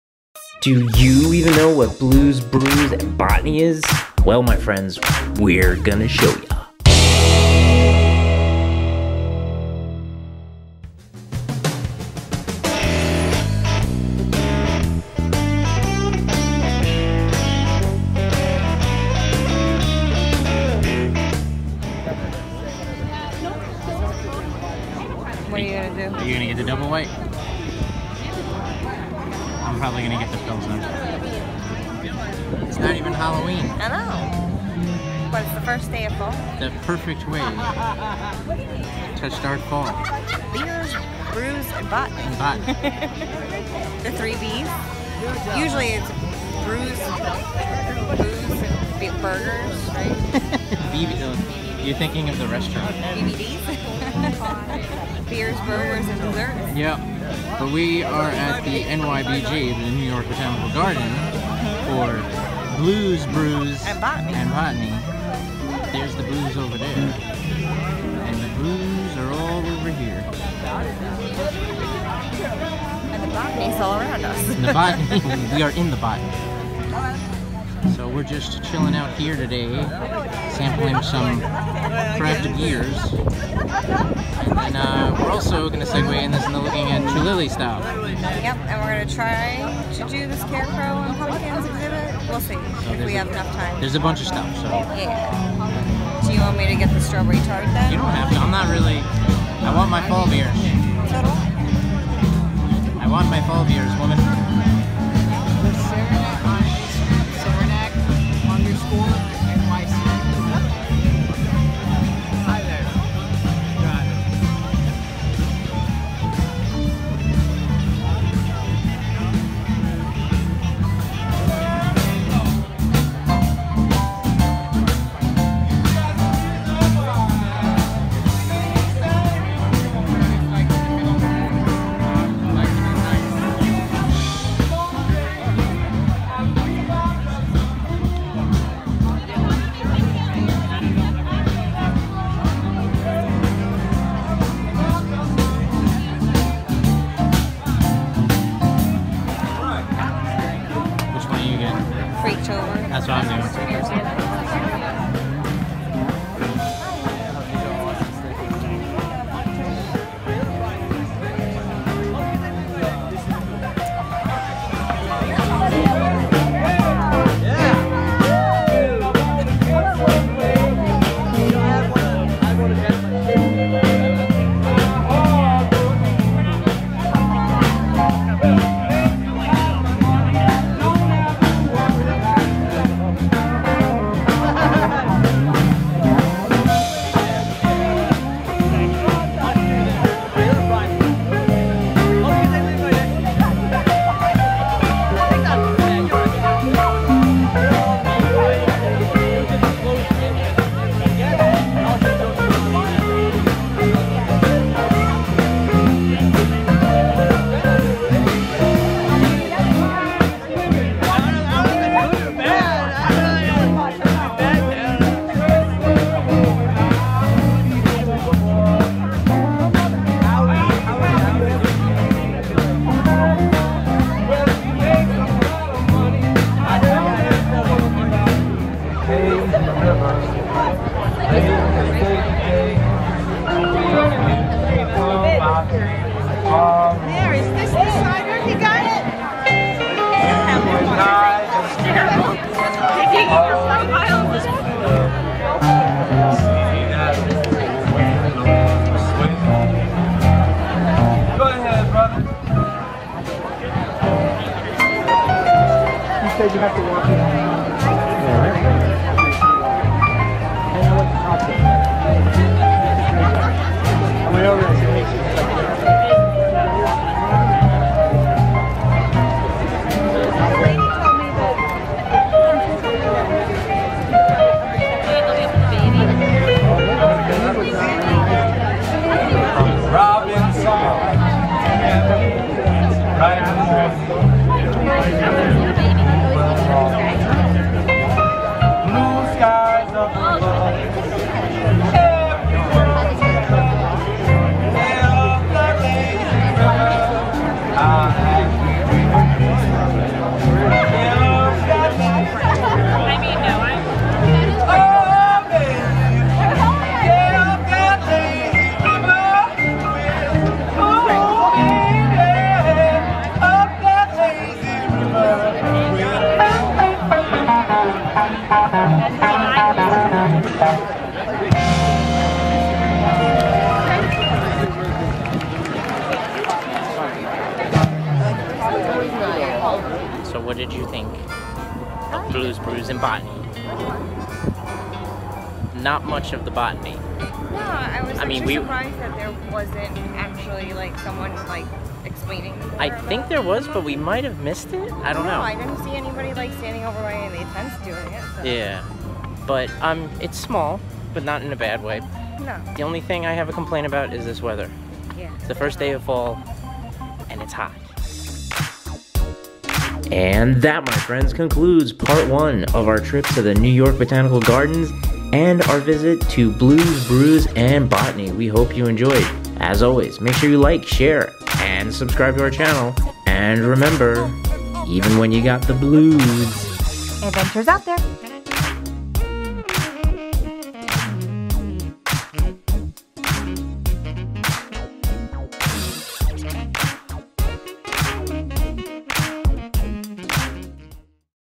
NYBG Trip: Part 1: 2nd Annual Blues Brews & Botany
We check out the 2nd Annual Blues, Brews, and Botany at the New York Botanical Garden. Join us for a flight of beers on the beautiful conservatory lawn as we celebrate the first day of fall with music by Paul Josephs Blues Band ) and botanical-inspired fun. Adventure is Out There!